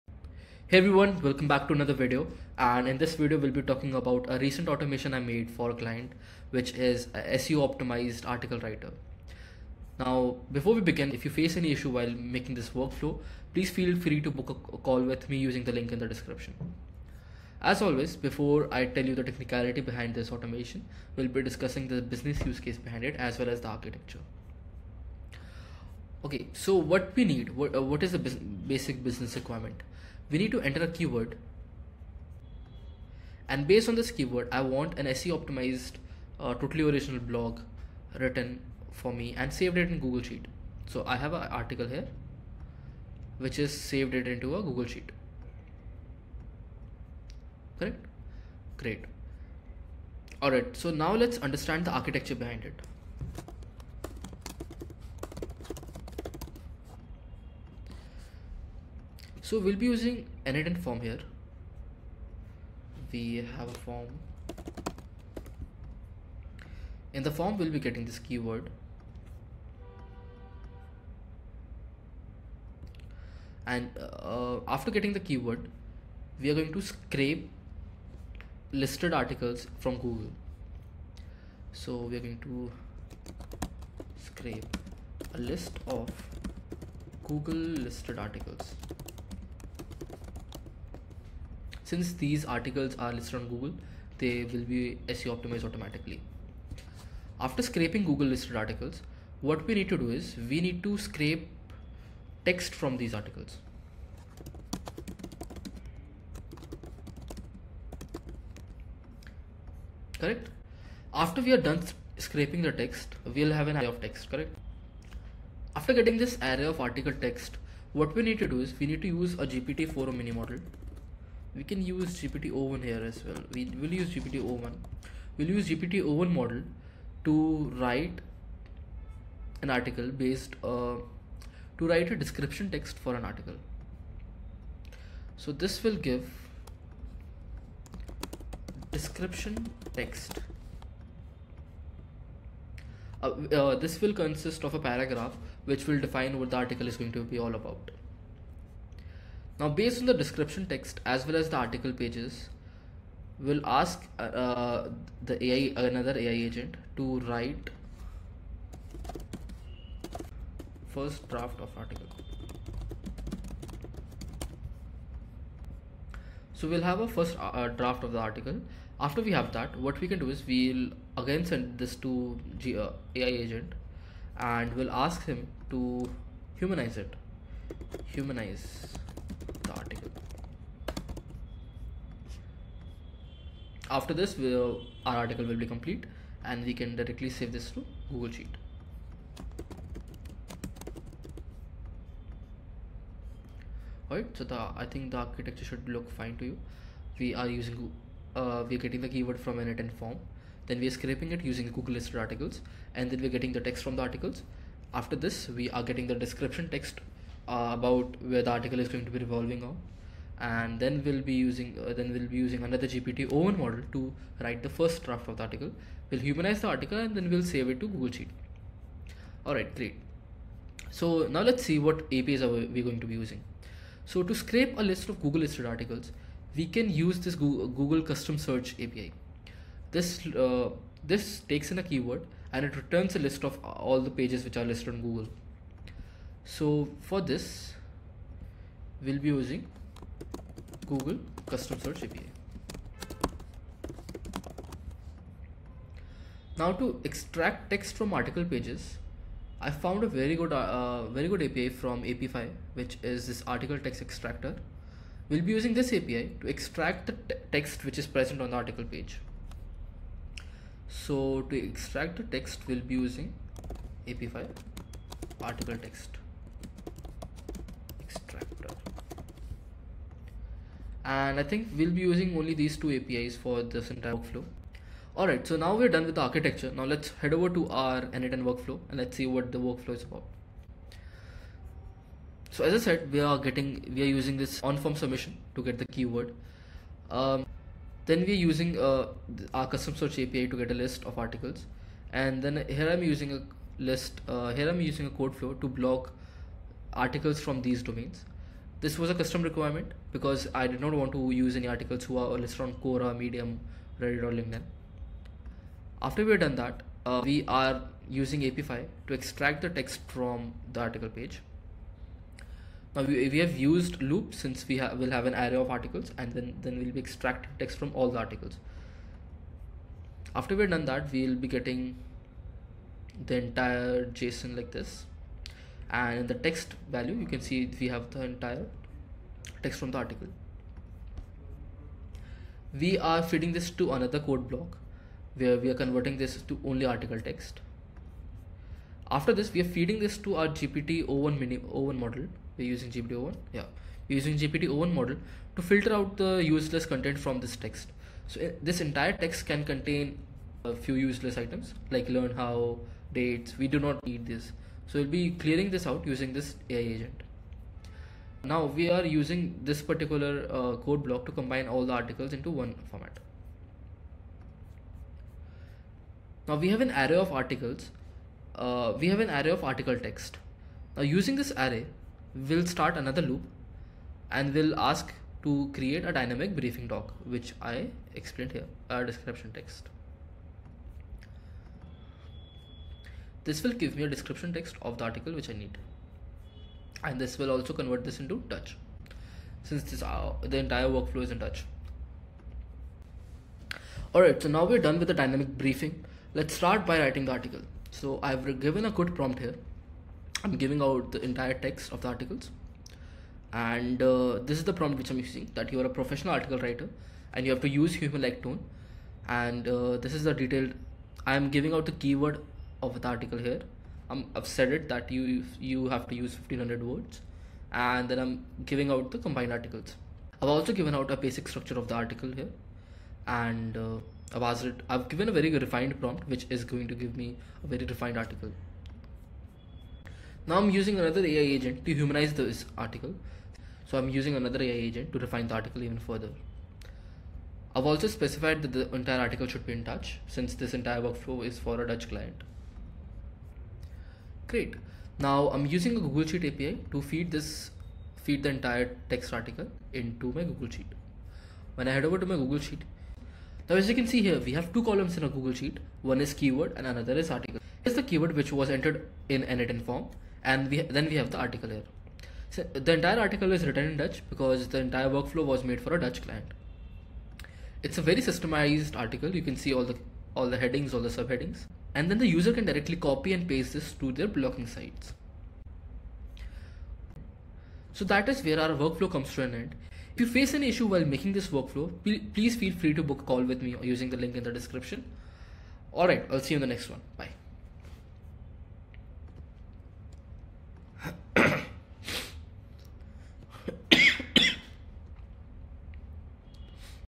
Automate Blog Writing with SEO AI Agent on N8N (Free Template)
In this tutorial, I’ll show you how to build an AI-driven system that finds trending articles, extracts key insights, and generates SEO-optimized content—all without writing a single line of code.

🔥 What You’ll Learn:
✅ Using Google Custom Search API to find trending articles in your niche.

✅ Extracting article content using Apify’s Article Text Extractor for analysis.

✅ Automating keyword research & tone analysis with GPT o1.
✅ Generating high-quality, SEO-optimized blog posts automatically.
✅ Storing and managing content in Google Docs seamlessly.

By the end of this tutorial, you’ll have a fully automated blogging system that saves hours of manual research and helps you rank higher on Google effortlessly.

👉 Ready to scale your content marketing? Watch now!

🔔 Become a member for exclusive benefits!
✅ Includes detailed responses to all your comments!